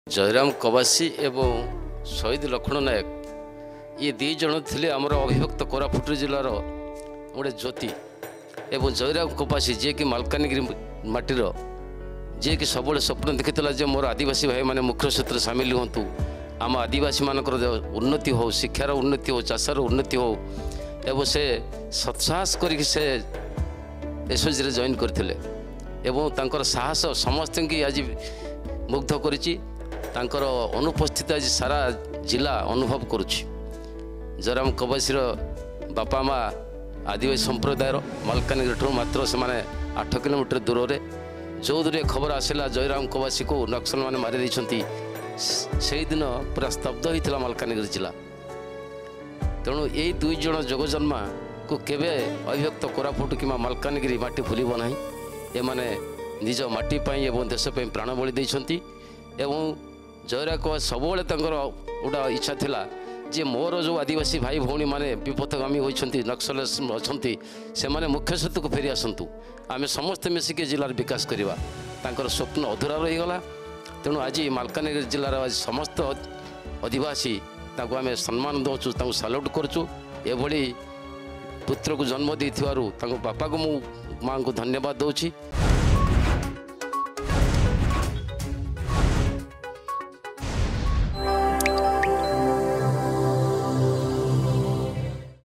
ସହିଦ୍ ଚାହୁଁଥିଲେ ଧ୍ବସ୍ତ ହେଉ ଆଦିବାସୀ ନକ୍ସଲ ସଂପର୍କ ।
ମାଲକାନଗିରିର ଆଦିବାସୀ ମାନଙ୍କ ଗୌରବ ସହିଦ୍ ଜୟରାମ କବାସି । ଆଦିବାସୀମାନେ ସବୁବେଳେ ଦେଶପ୍ରେମୀ । ସହିଦ ଲକ୍ଷ୍ମଣ ନାୟକଙ୍କ ଭଳି ଜୟରାମଙ୍କୁ କେବେବି ଭୁଲିବନି ଅବିଭକ୍ତ କୋରାପୁଟ ଜିଲ୍ଲା ।